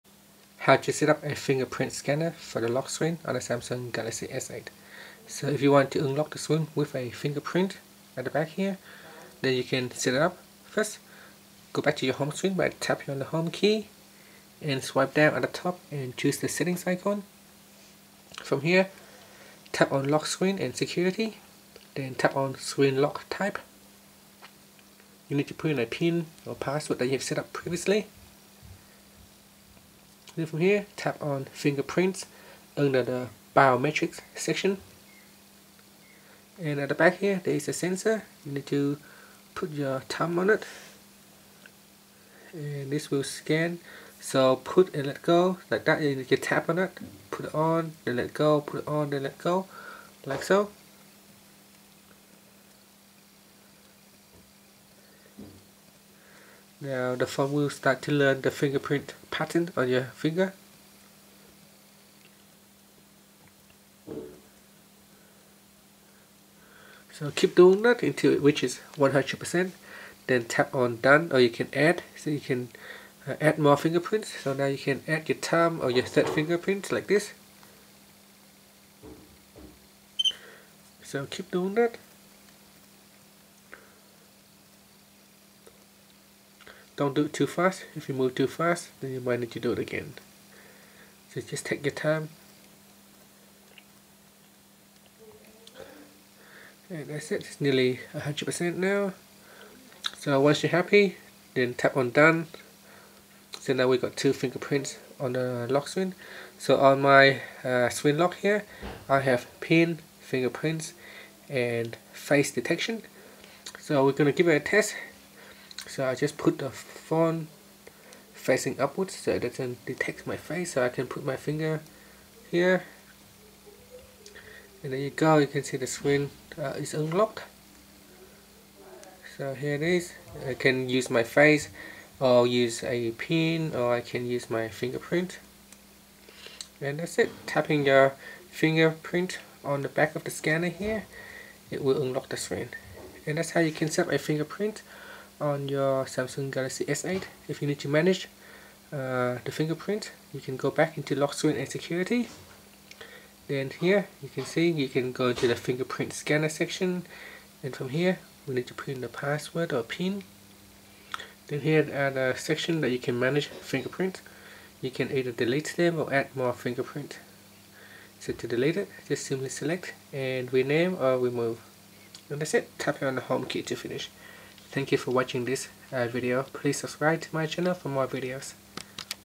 Samsung Galaxy S8: How to Setup Fingerprint Scanner for Lock Screen
Learn how you can setup the fingerprint scanner for lock screen on Samsung Galaxy S8.